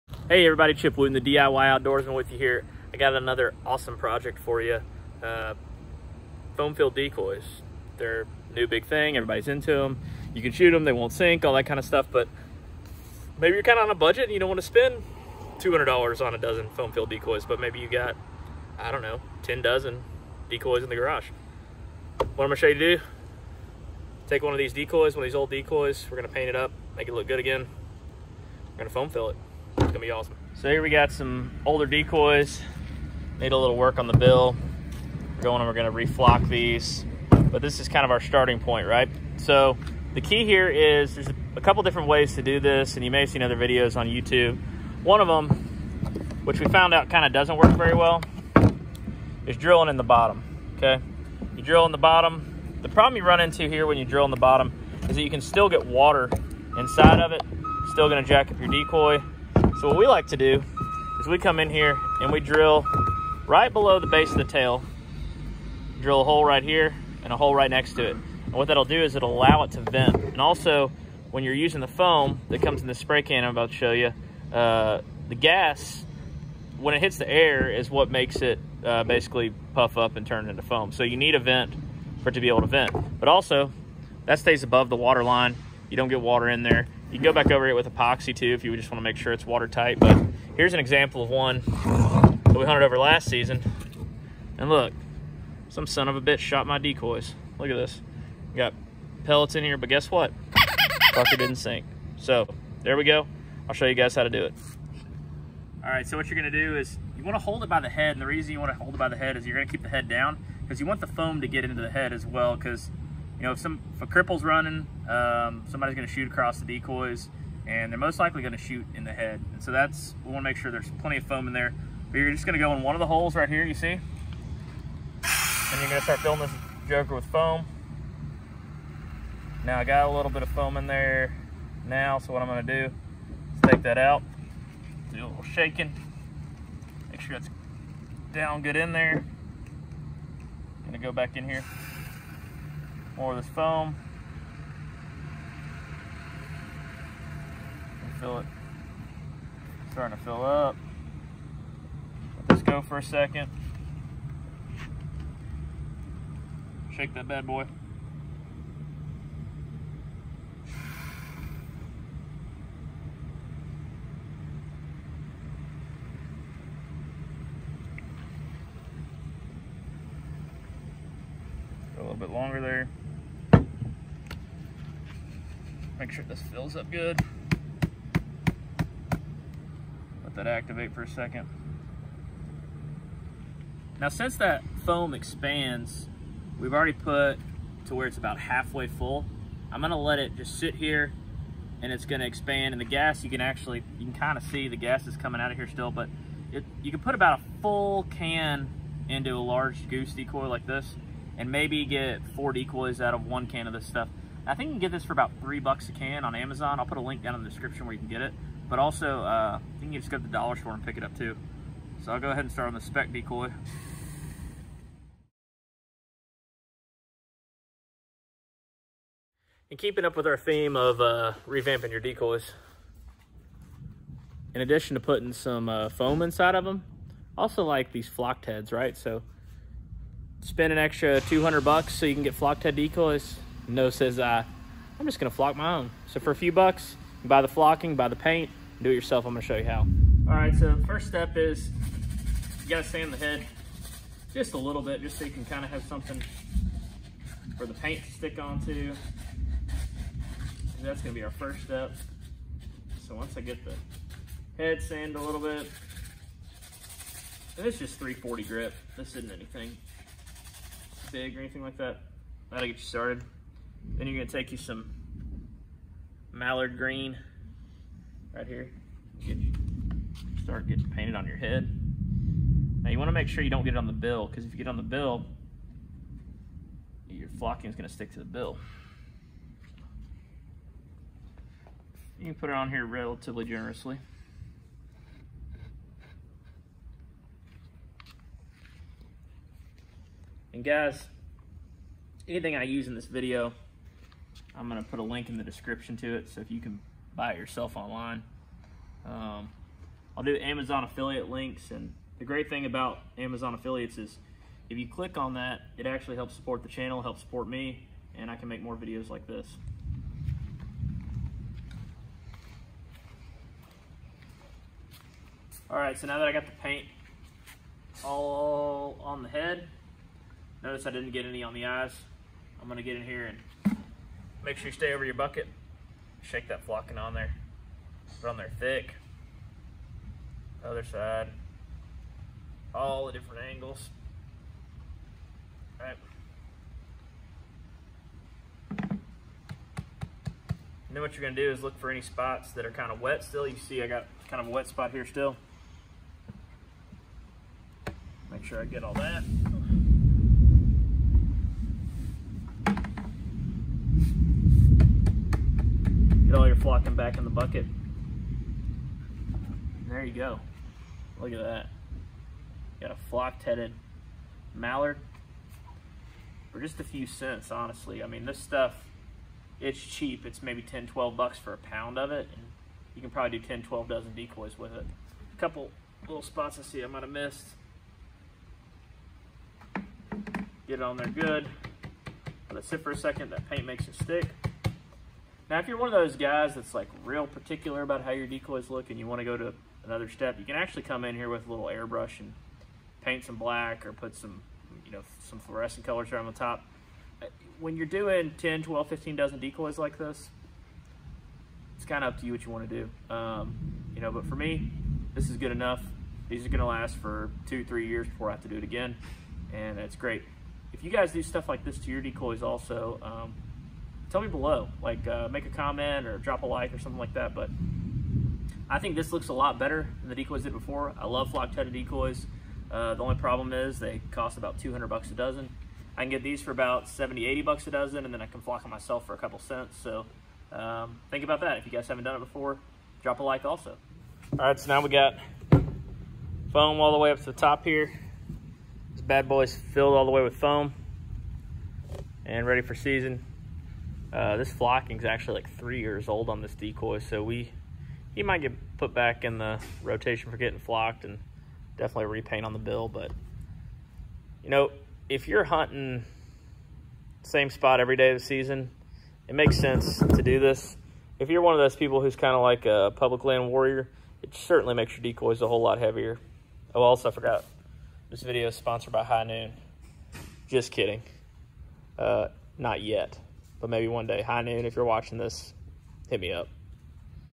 How to Flock Duck Decoys
Season is just a few months away now and we have some work to do on the decoys.
Below are some of the products used in this video.

Decoy Flocking
Mallard Green Decoy Paint
Foam Insulation
Paint Brushes
Silicone Sealant
GoPro Hero 9
GoPro Hero 8